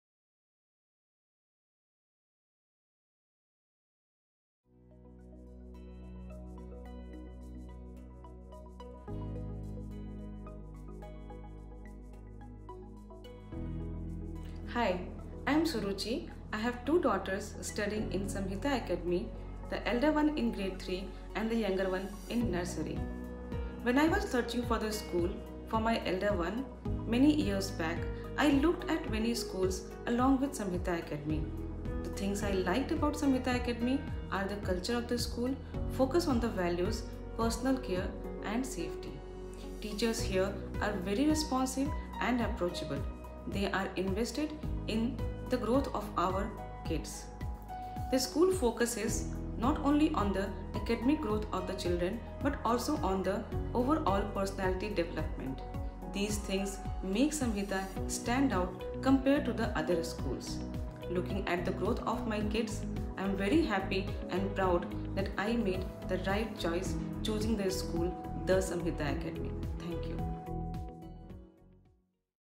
Parent Testimonials by Ms. Suruchi Srivastav | The Samhita Academy
Thank you, Ms. Suruchi Srivastav, for such wonderful words of appreciation. We are delighted to be a part of this wonderful educational journey, and acknowledgments always motivate us to be better than yesterday..

The Samhita Academy